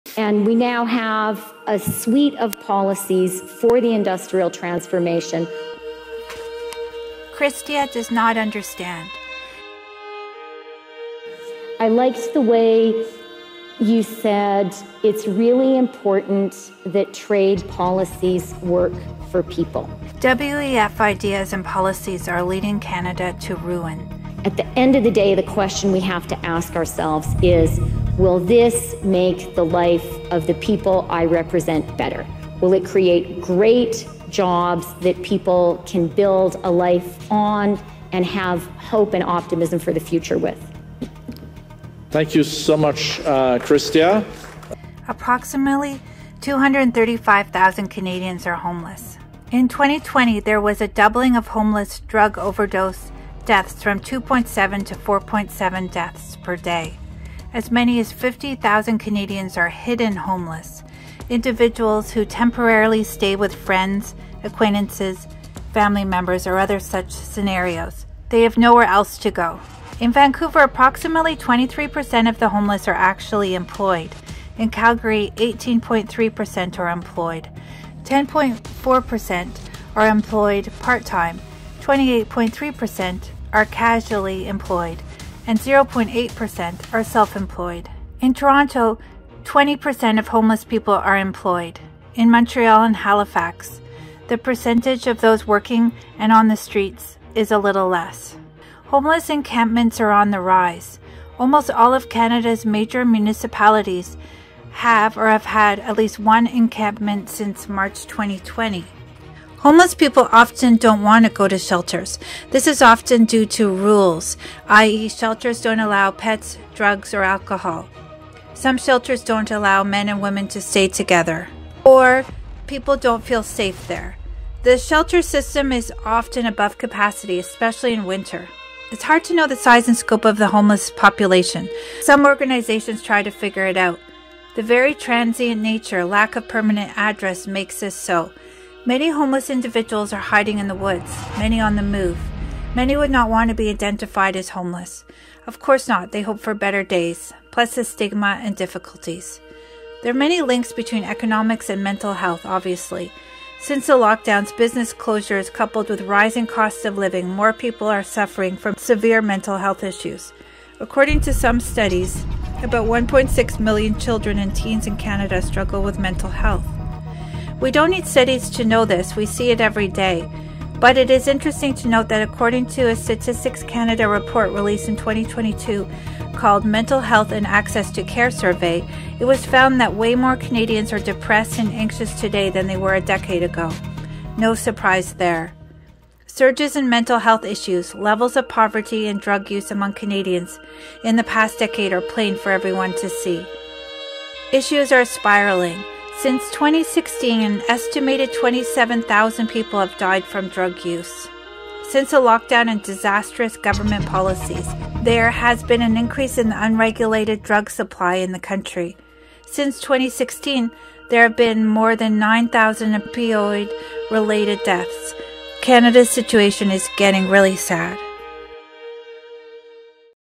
Please Pray for Canada's Homeless & Economic Situation
homeless With the Deputy Prime Minister Chrystia at the World Economic Forum Trade and Investment Panel this week (January 18, 2024), the question is this: what is the best way forward for the Canadian people? Please pray for all Canadians. The WEF way is not the Canadian way.